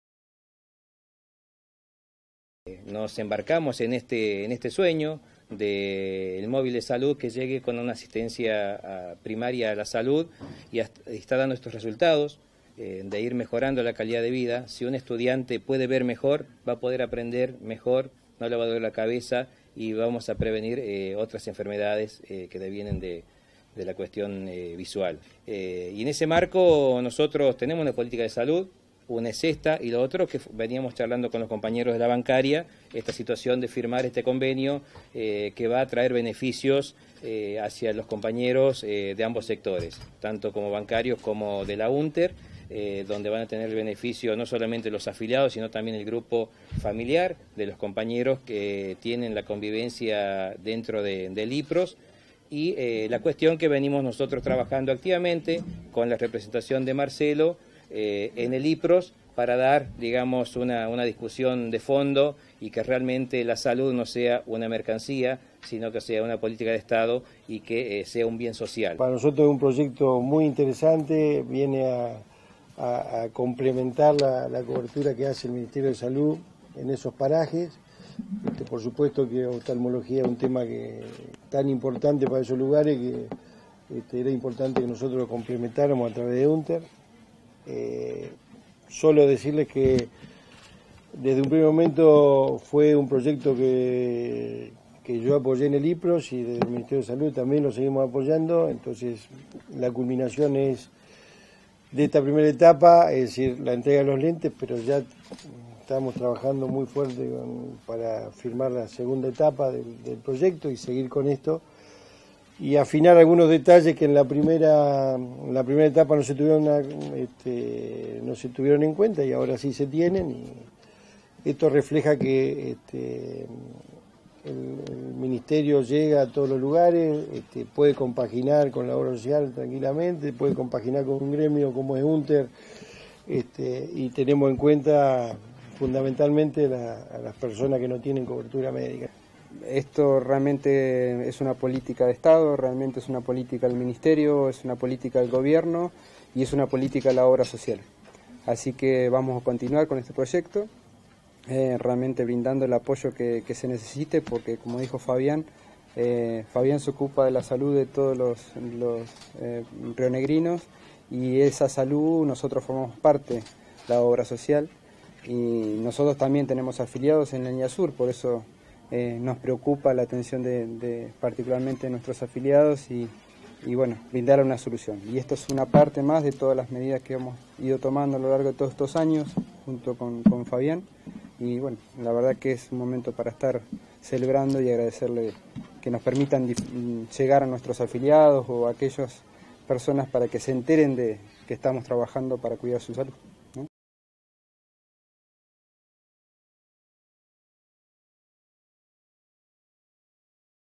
Viedma - Convenio por el móvil de Salud de UnTER
El ministro de Salud, Fabián Zgaib, anunció hoy la
Continuidad del apoyo de su cartera y del IPROSS al proyecto Móvil de Salud del gremio docente UNTER. A través de este plan, se refuerza la atención primaria que brinda el Gobierno provincial a pobladores de parajes y pequeñas localidades. El proyecto Móvil de Salud  se puso en marcha en 2014. A través del mismo, se  brinda atención médica, odontológica y oftalmológica.